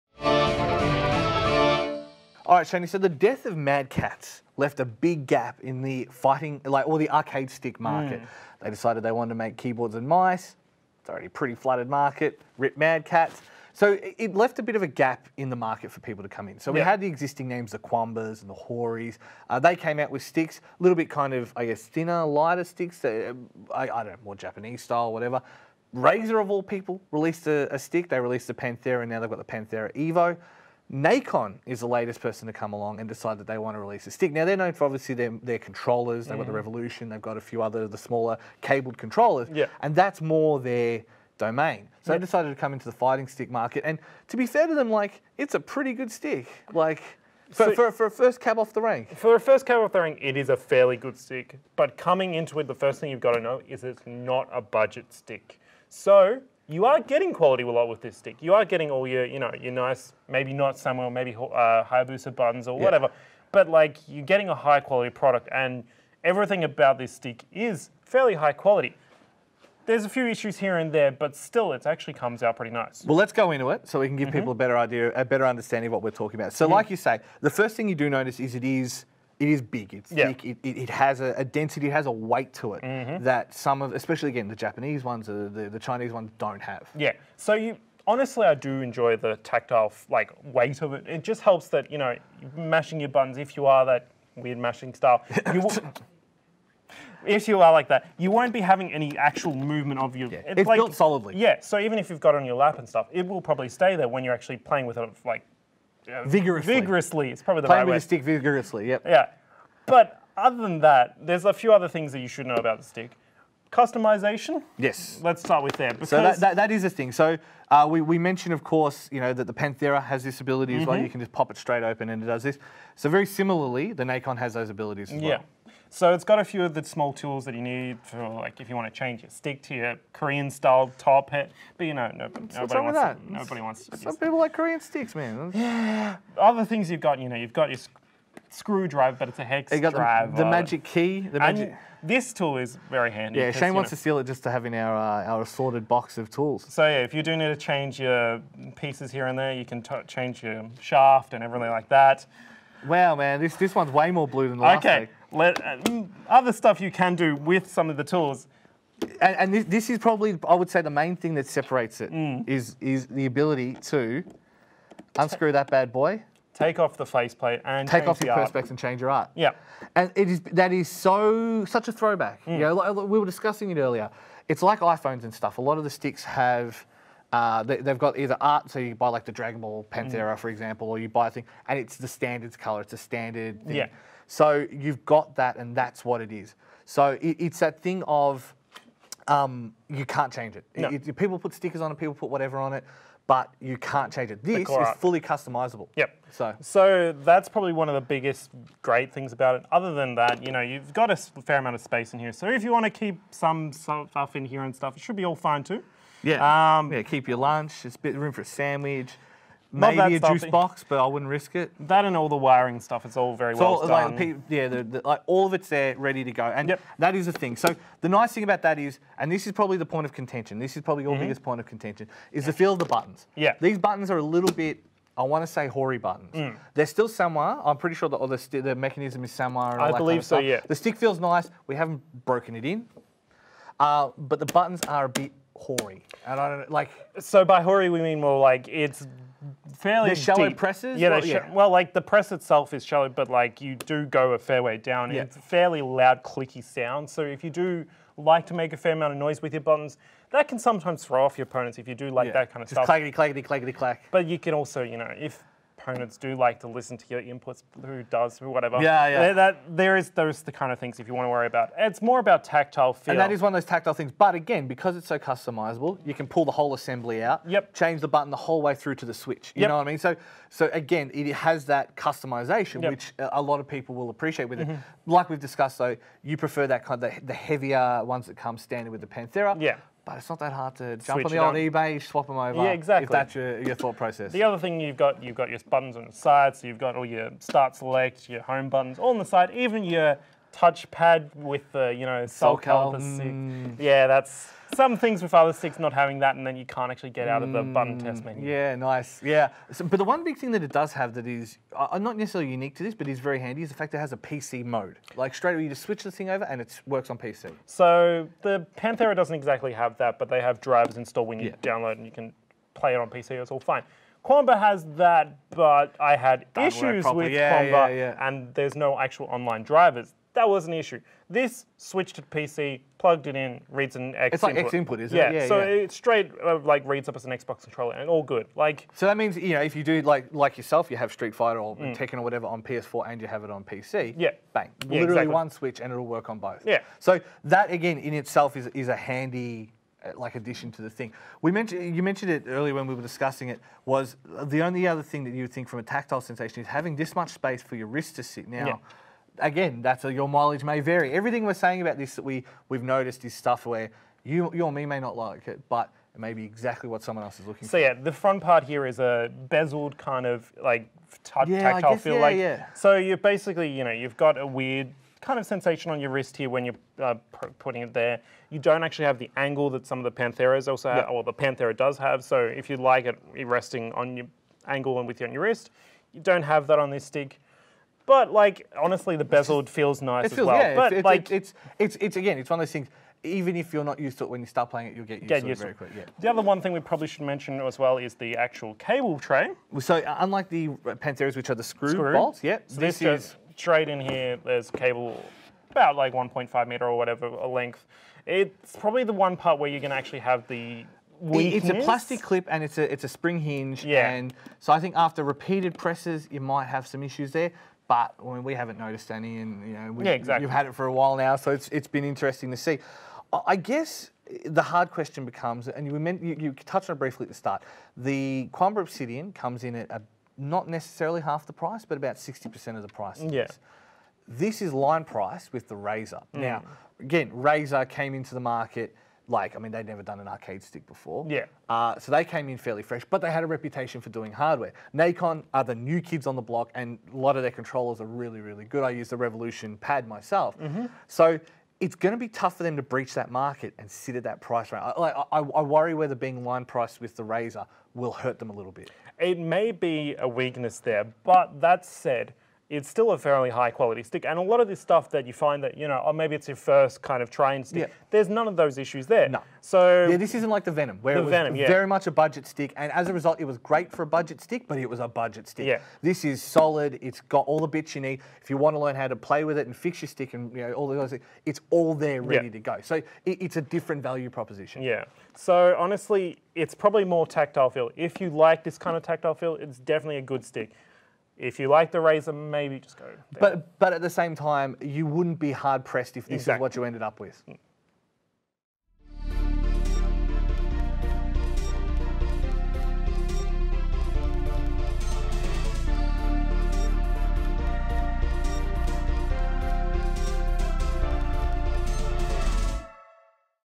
Nacon Daija Arcade Stick - NEW GAME PLUS TV REVIEWS
In the world of fighting game sticks, a new challenger has entered the arena! We check out the Nacon Daija and see how it stacks up to the competition.